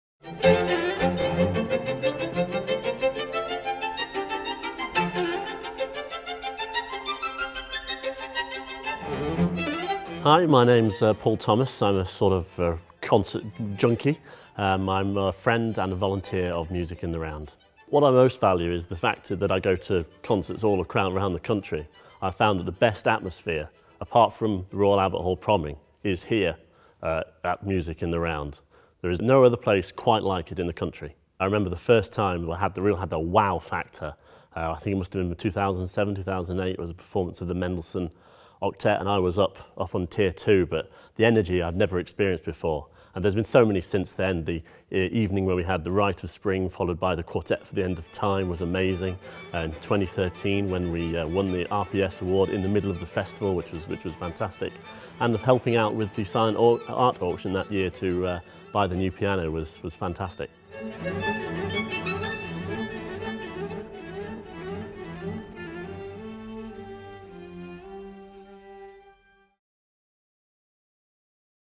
30 short films celebrating Music in the Round at 30 - Paul Thomas
In celebration of Music in the Round's 30th birthday in 2014, 30 different players, supporters, audience members, music students, teachers, broadcasters and colleagues share something that matters to them about live classical music, Music in the Round and its education and audience engagement programme, Music in the Community. Filmed in our Sheffield home, the Crucible Studio Theatre.